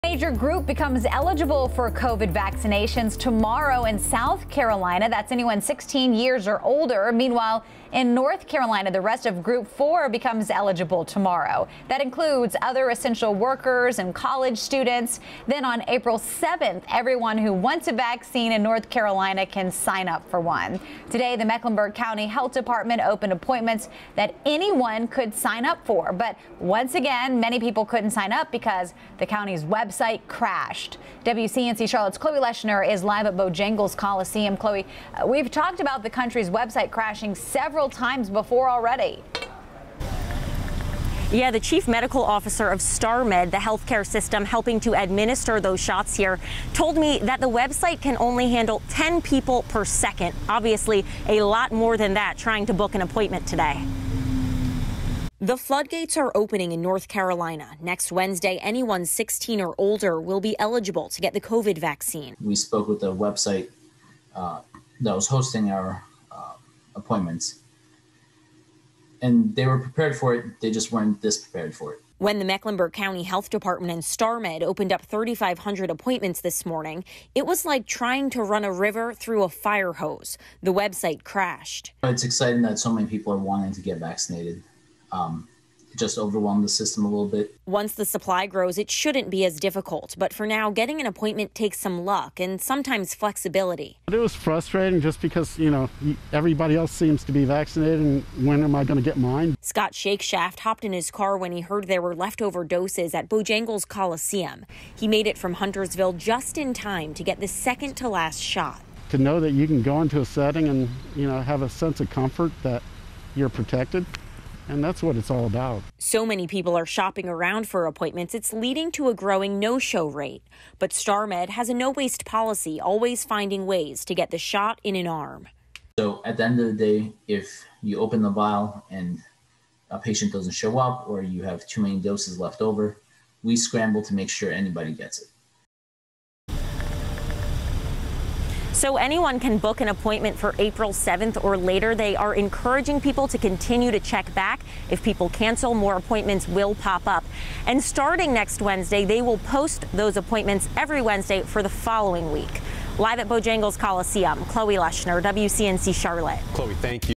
Registration system crashes as Mecklenburg Co. opens vaccine appointments
Mecklenburg County's COVID-19 vaccine registration system was overwhelmed by patients Tuesday as the county opened appointments for all adults age 16 and older.

At 8:30 a.m. Tuesday, Mecklenburg County Public Health opened appointments for what will be Group 5 vaccinations. The system was quickly overloaded by users trying to schedule their appoint. In a tweet, the county asked for patience as they were experiencing "technical difficulties."

StarMed, the health care provider administering the vaccines at Bojangles Coliseum, said they simply had too many patients trying to schedule at once. Arin Piramzadian, the chief medical officer for Starmed, told WCNC Charlotte's Chloe Leshner their system was capable of handling up to 10 patients per second register and the demand was much greater than that to crash the system.

"We spoke with the website that was hosting our appointments and they were prepared for it, they just weren't this prepared for it," Piramzadian said. "It's exciting that this many people want to get vaccinated. it just overwhelmed the system a little bit."